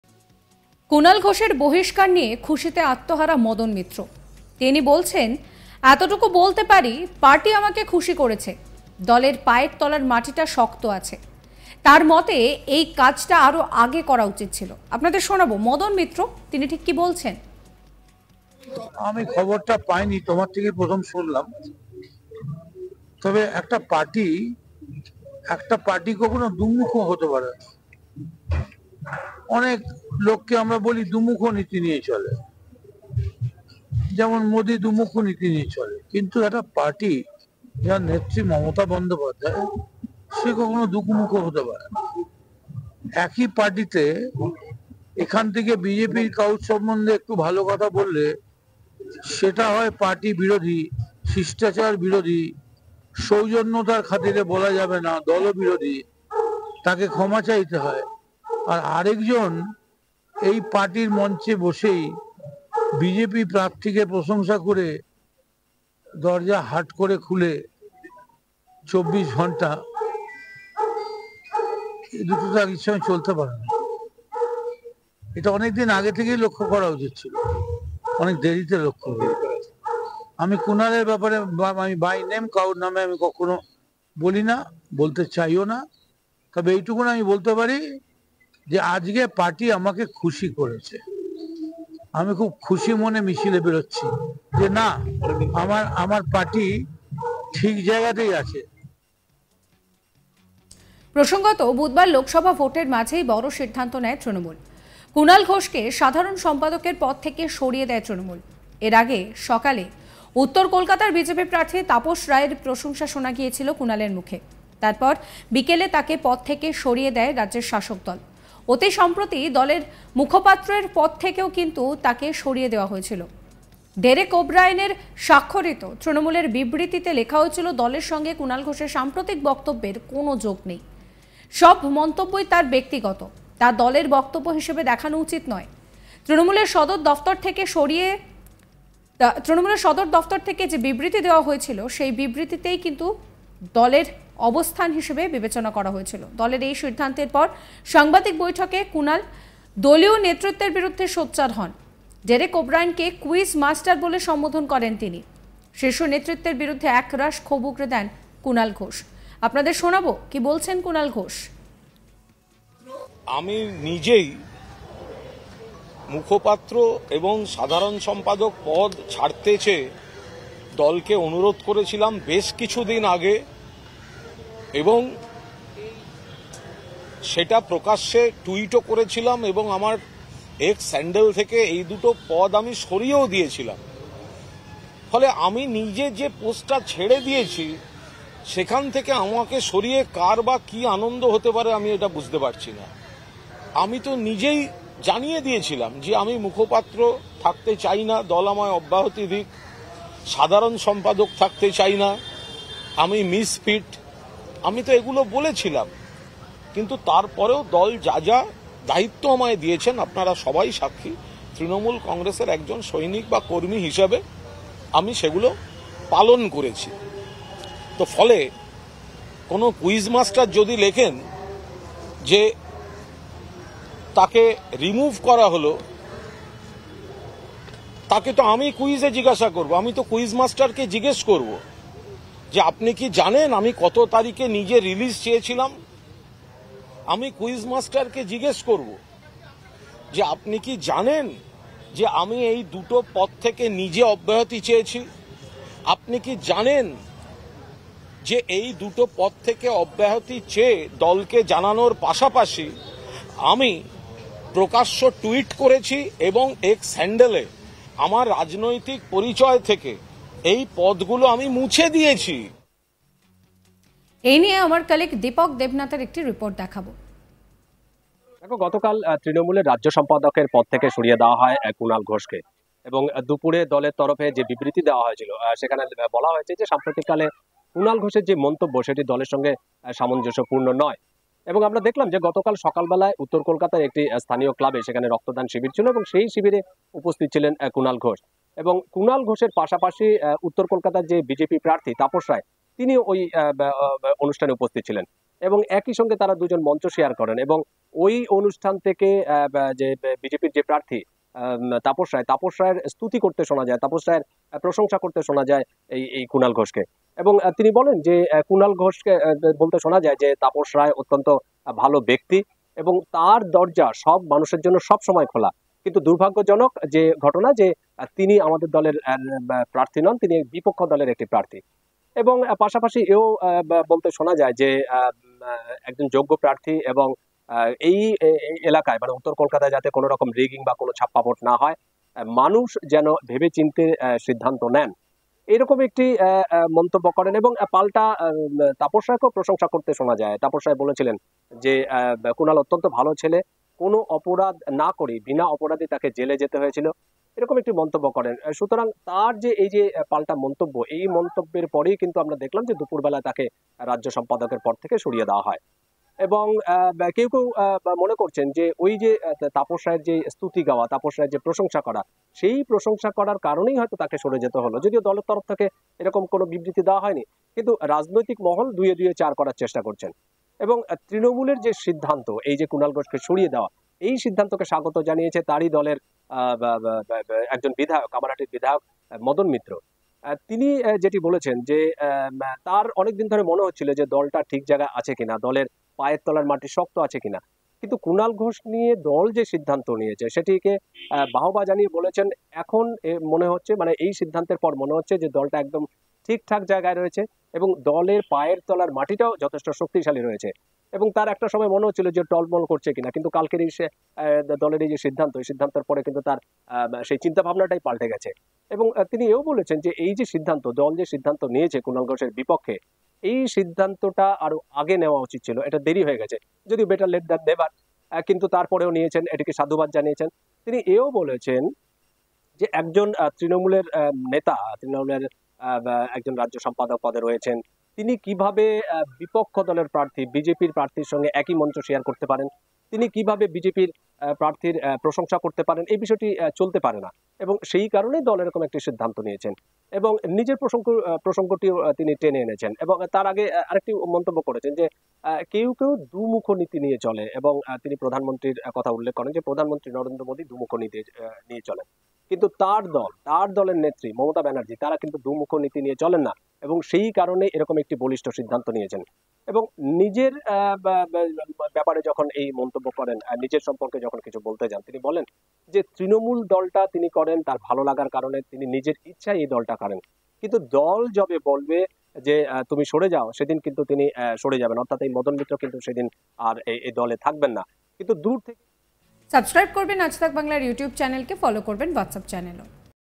​Breaking: 'আরো আগে উচিত ছিল', কুনালের বহিষ্কারে মদন মিত্রের খুশি দেখুন | Lok Sabha Elections| Kunal
প্রসঙ্গত, বুধবার লোকসভা ভোটের মাঝেই বড় সিদ্ধান্ত নেয় TMC। কুণাল ঘোষকে সাধারণ সম্পাদকের পদ থেকে সরিয়ে দেয় তৃণমূল। এর আগে সকালেই উত্তর কলকাতার বিজেপি প্রার্থী তাপস রায়ের প্রশংসা শোনা গিয়েছিল কুণালের মুখে। তার পর বিকেলে তাঁকে পদ থেকে সরিয়ে দিল রাজ্যের শাসক দল। অতিসম্প্রতি দলের মুখপাত্রের পদ থেকেও সরিয়ে দেওয়া হয়েছিল। ডেরেক ও'ব্রায়েনের স্বাক্ষরিত তৃণমূলের বিবৃতি লেখা হয়েছে,'দলের সঙ্গে কুণাল ঘোষের সাম্প্রতিক বক্তব্যের কোনও যোগ নেই। সব মন্তব্য তাঁর ব্যক্তিগত। তা দলের বক্তব্য হিসেবে দেখা উচিত নয়। তৃণমূলের সদর দফতর থেকে দেওয়া বিবৃতিই দলের অবস্থান হিসেবে বিবেচনা করতে হবে।